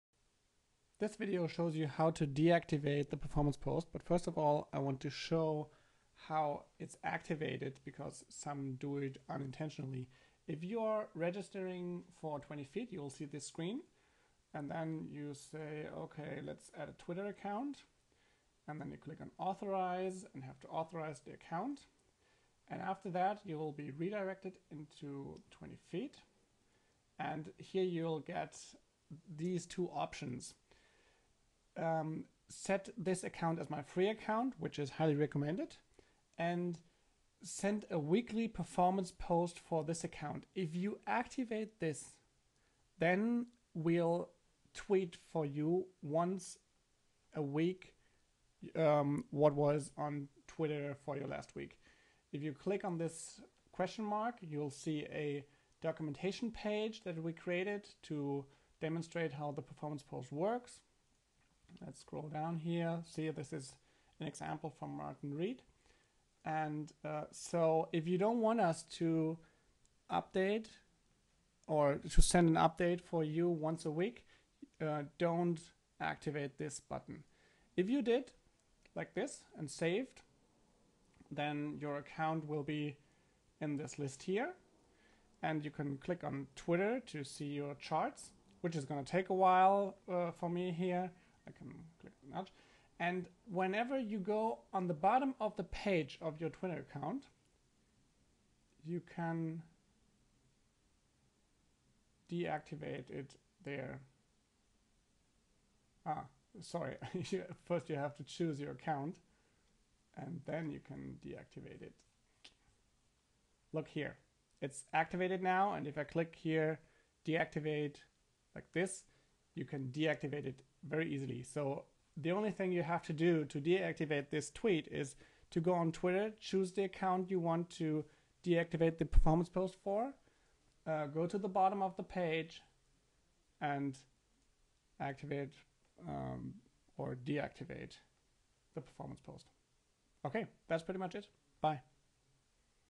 How to deactivate the performance post for twitter from TwentyFeet
- Video-Tutorial on why your performance post is sent out and how to deactivate it.